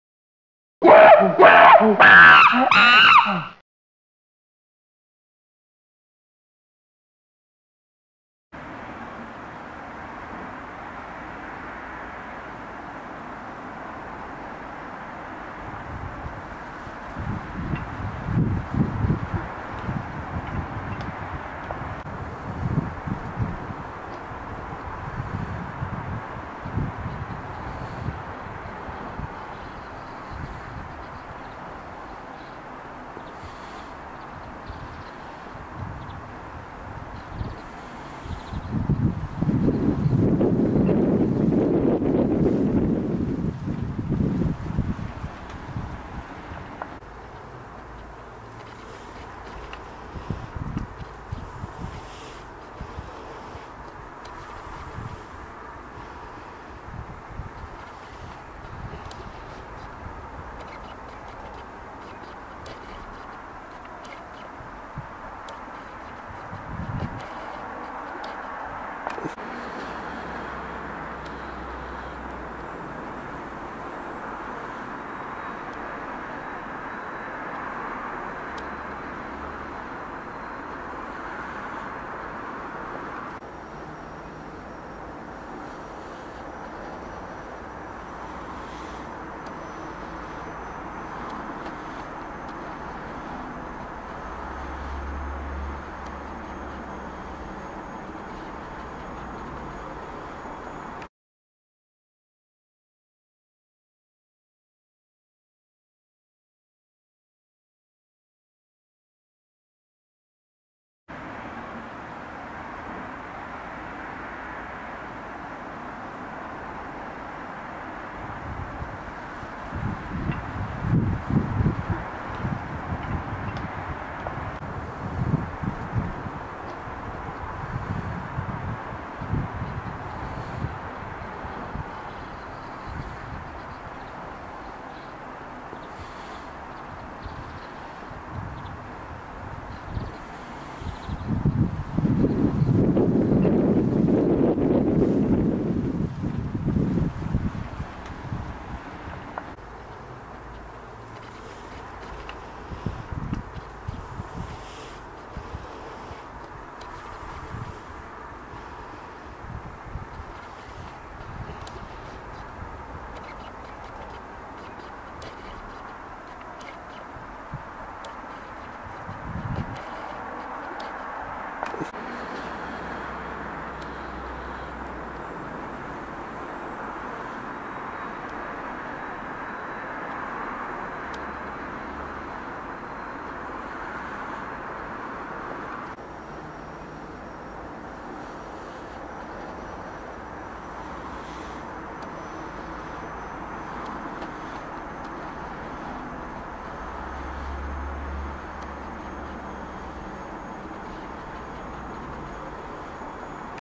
Film Look - Test
So, I have been putting the Sony SLT a58 DSLR through it's paces and have been trying some colour correction to give a film look using Sony Movie Studio Platinum 12.